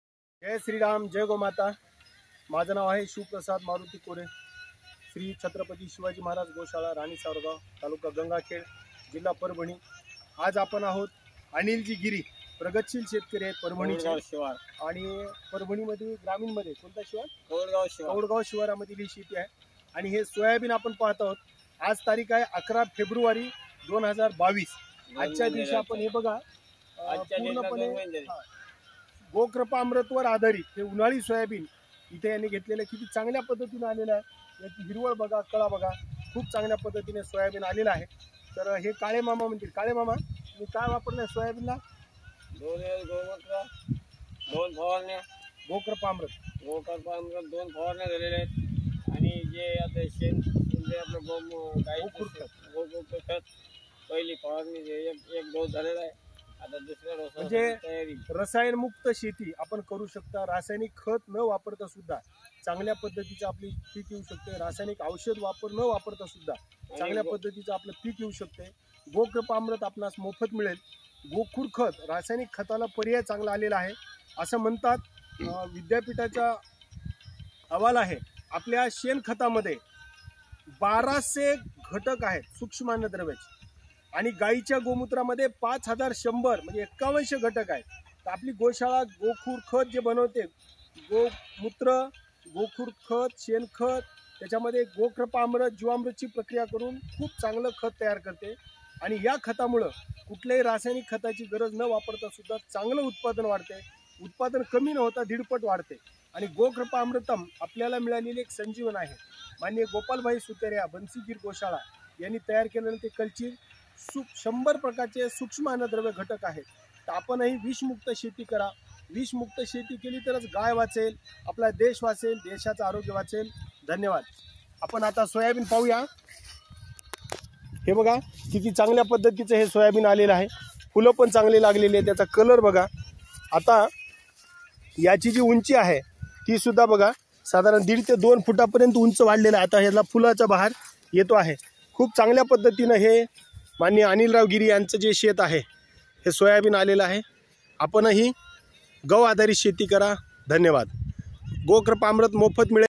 गोकृपा अमृत गोखुर खत वापरुन आलेले सोयाबीन. श्री अनीलजी गीरी परभणी.9421389861
गोकृपा अमृत सर्व शेतकरी बंधूंना गोमातेकडून मिळालेली देणगी आहे. श्री अनिल जी गिरी परभणी यांच्या शेतात  गोकृपा व गोखुर कामधेनु खत वापरून आलेले विषमुक्त सोयाबीन श्री छत्रपती शिवाजी महाराज गोशाळा राणीसावरगाव तालुका गंगाखेड जिल्हा परभणी.8421737513